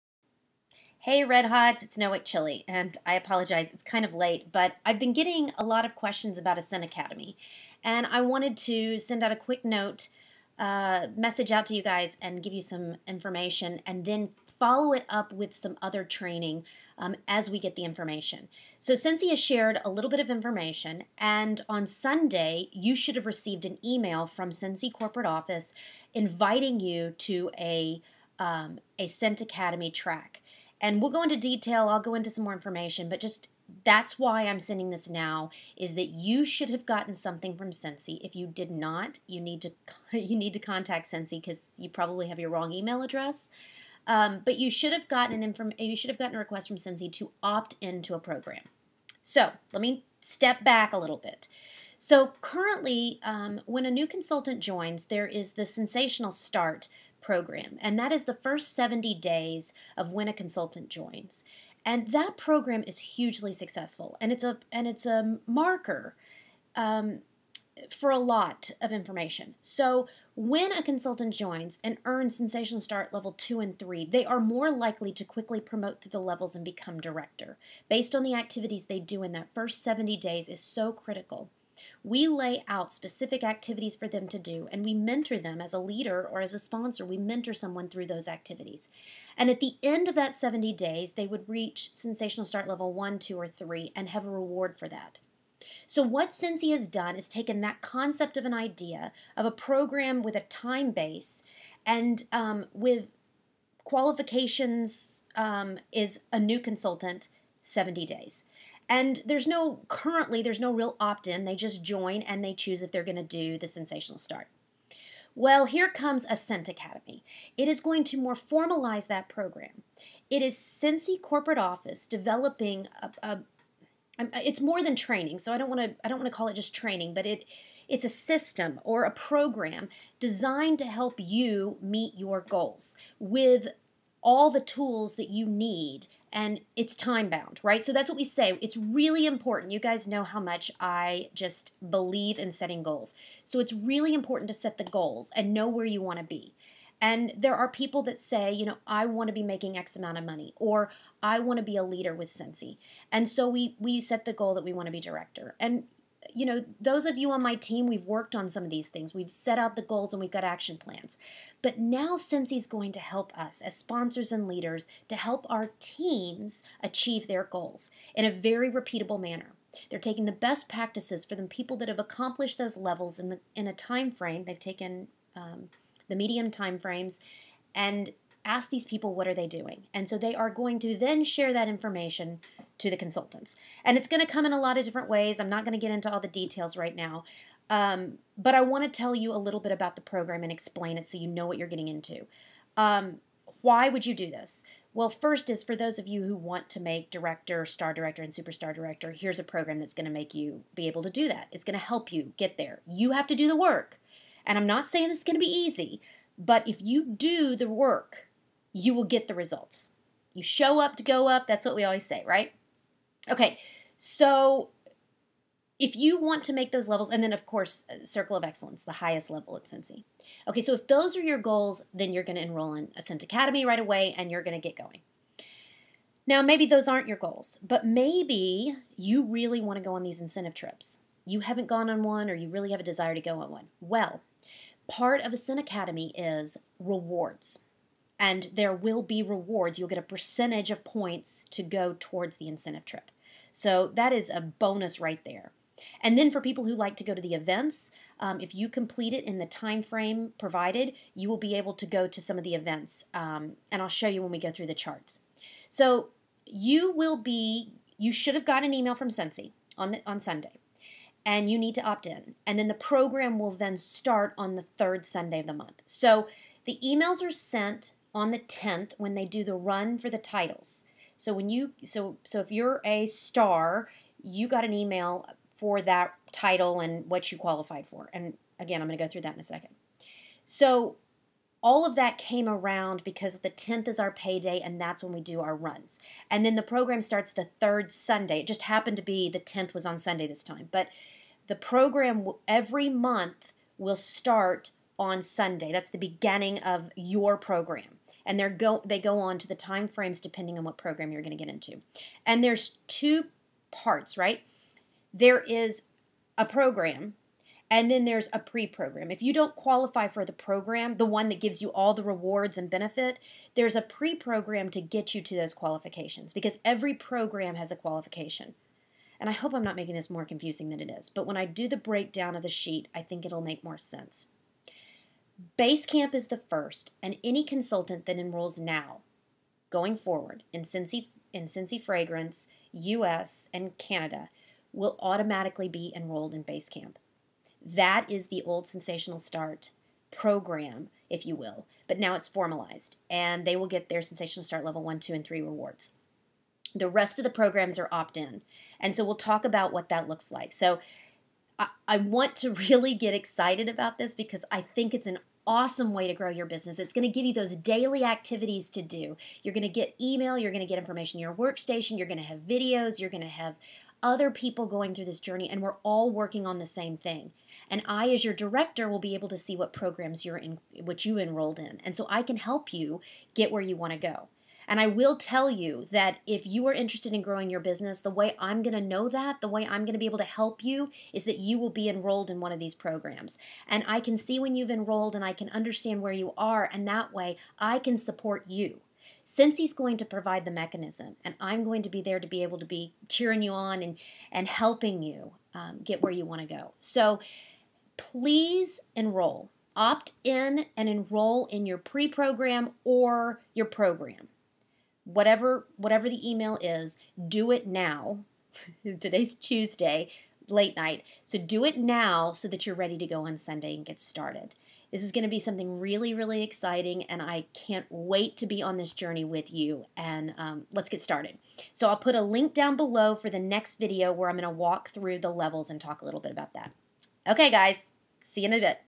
Scentsy Ascent Academy - Part 1 intro
Providing a quick overview of the new Scentsy program called Ascent Academy. In part 2 I will walk through the actual chart with the levels & prerequisites.